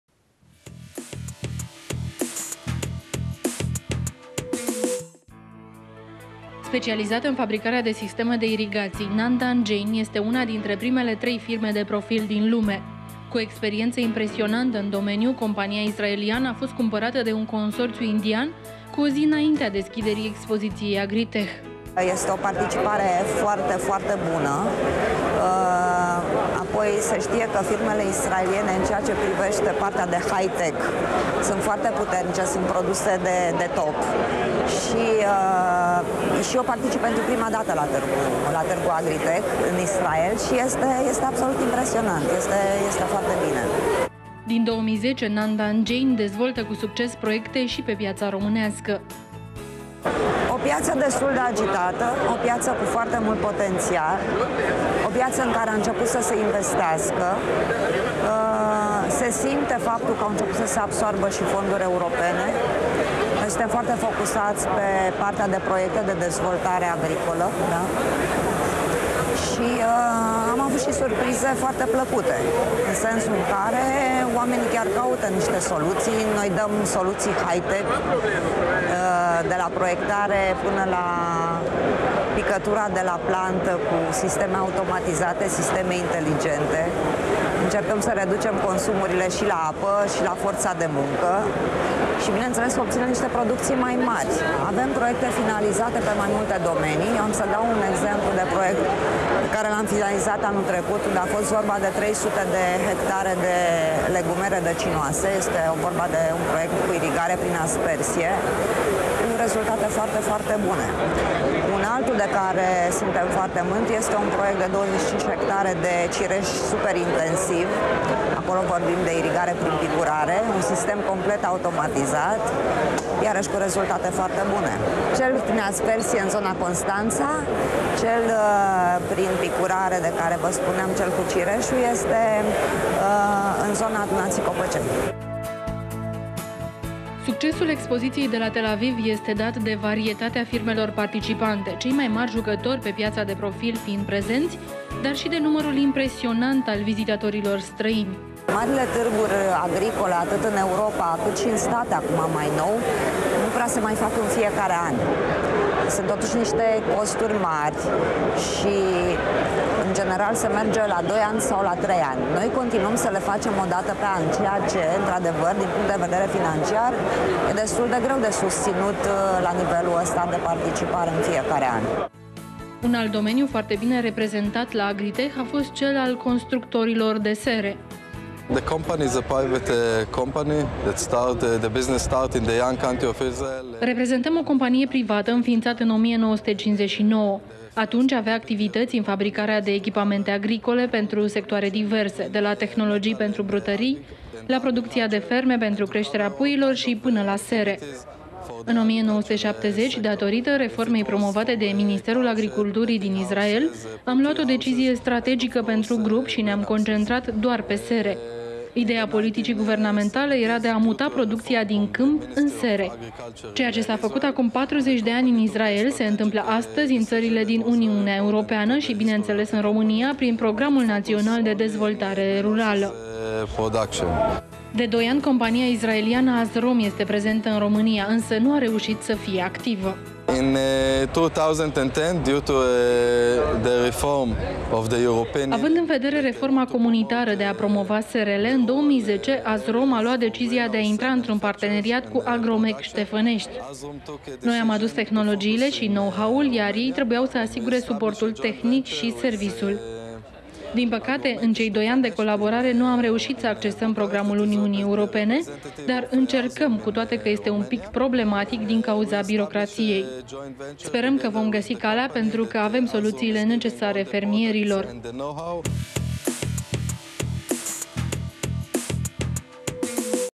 EXPOZANŢI AGRITECH 2012
Specializată în fabricarea de sisteme de irigaţii, Naandanjain este una dintre primele trei firme de profil din lume. Cu o experienţă impresionantă în domeniu, compania israeliană a fost cumpărată de un consorţiu indian cu o zi înaintea deschiderii expoziţiei Agritech.
Succesul expoziţiei de la Tel Aviv este dat de varietatea firmelor participante - cei mai mari jucători pe piaţa de profil fiind prezenţi, dar şi de numărul impresionant al vizitatorilor străini.
Un alt domeniu, foarte bine reprezentat la Agritech, a fost cel al constructorilor de sere.
De doi ani, compania israeliană Azrom este prezentă în România, însă nu a reuşit să fie activă.